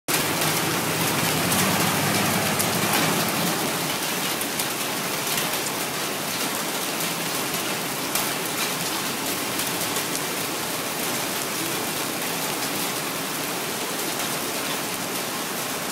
Round Rock hail
Andrea Penna captured video of what the hail looked like coming down in the Eagle Ridge part of Northeast Round Rock around 7:45 a.m.